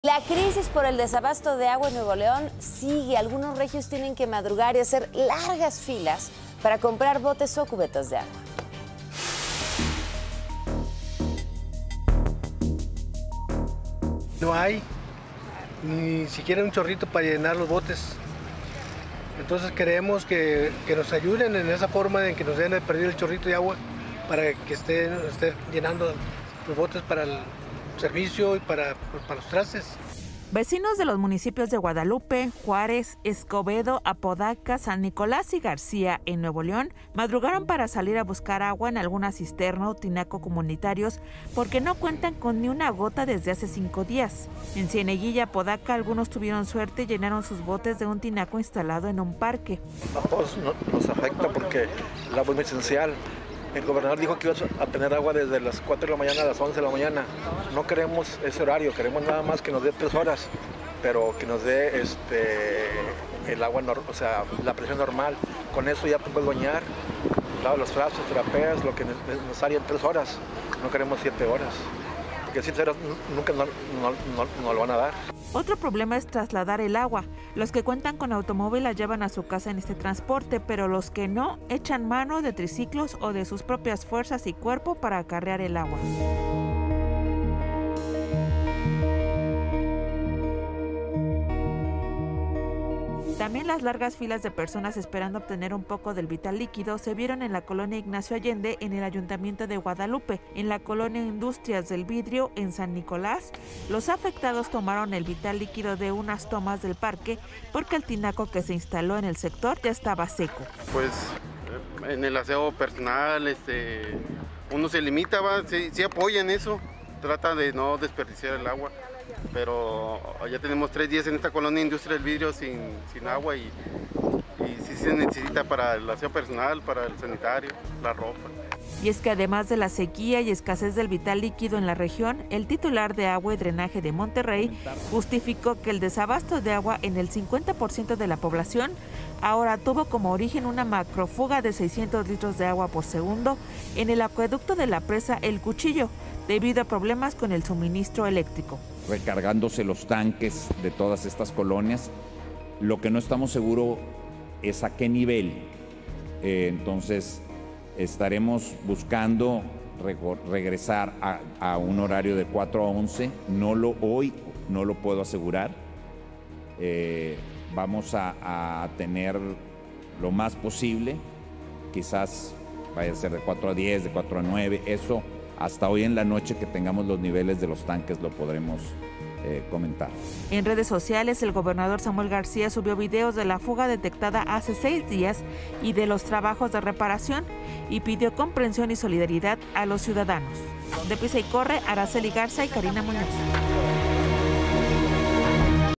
Así es el tormento para conseguir agua en Nuevo León
La desesperación por tener más de una semana sin agua potable en sus hogares obligó a las personas a encontrar la manera de llevar el líquido a su casa para utilizarla en las necesidades básicas.   21 junio de 2022

Te invitamos a ver el siguiente video:
Elecciones presidenciales en Colombia y el triunfo histórico de Gustavo Petro
//youtu.be/LCd8QbHag2E